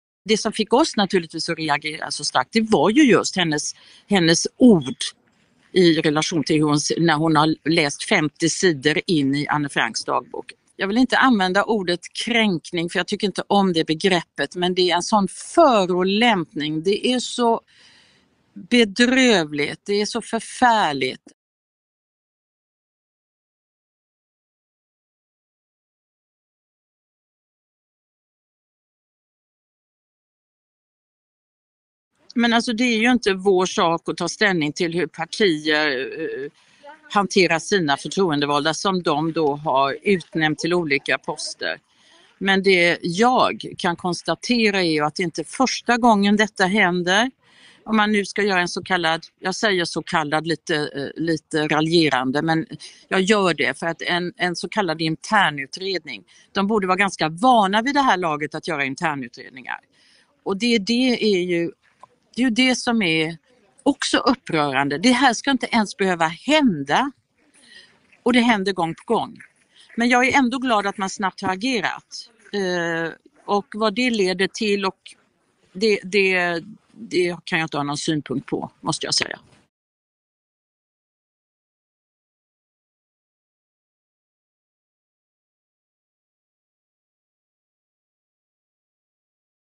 Judiska centralrådets ordförande: "Bedrövligt"
SD-profilen Rebecka Fallenkvist beskrev Anne Frank som ”kåtheten själv”.
Nu väljer partiet att ta henne ur tjänst.
Hör Judiska centralrådets ordförande.

Läs mer på aftonbladet.se